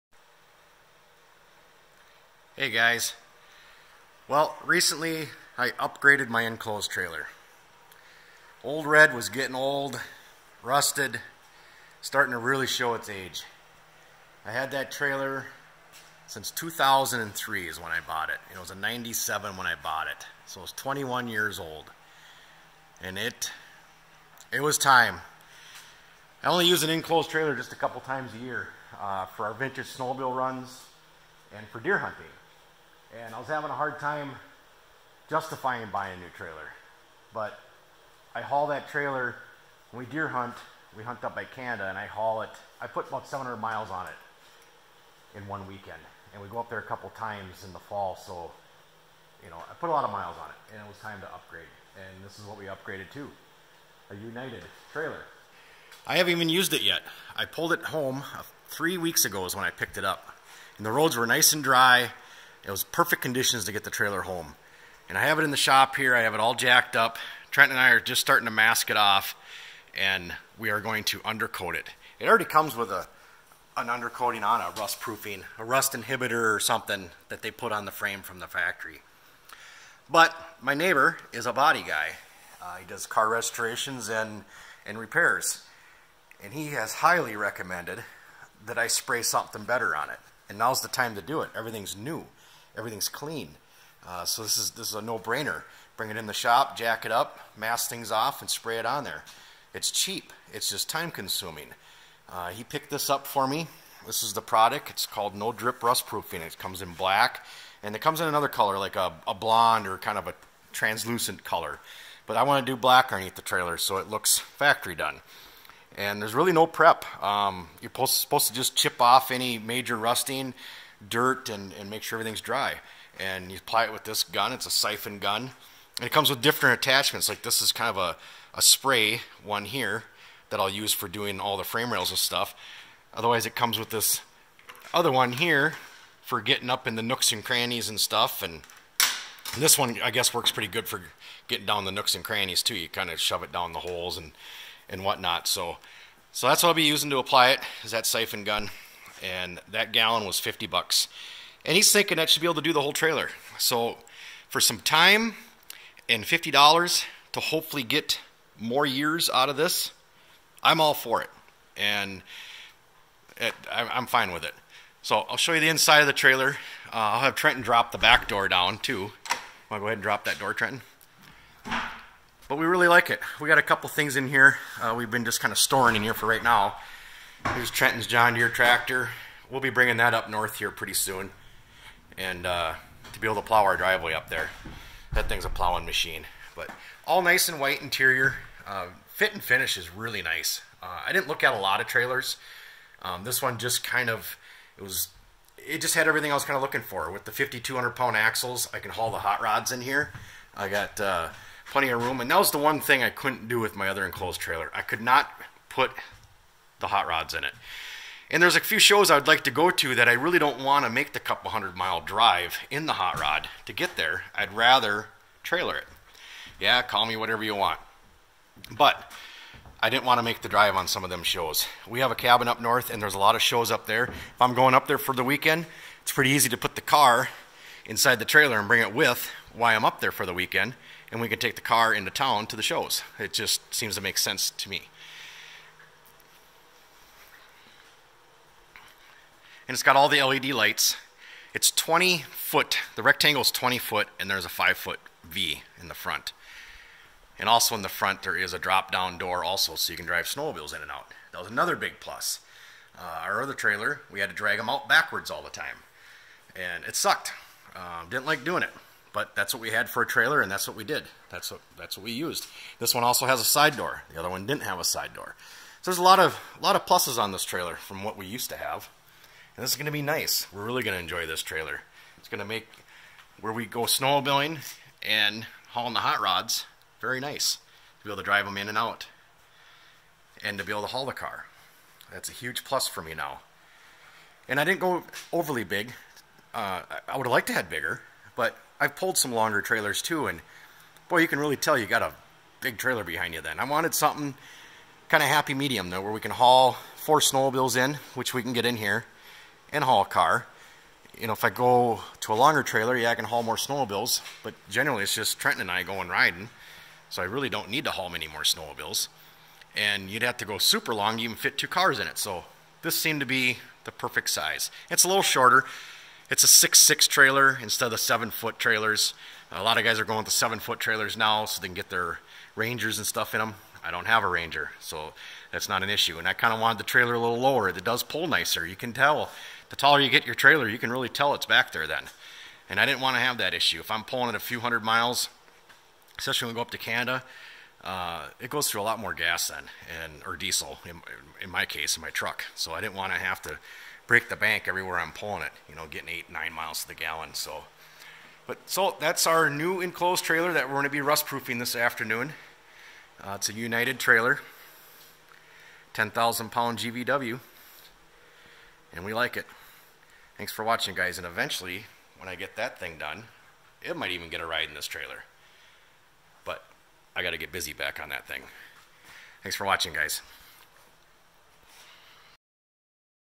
Enclosed Trailer review
This is my new 8.5 ft by 25 ft enclosed trailer. Using a rust proofing on the underside.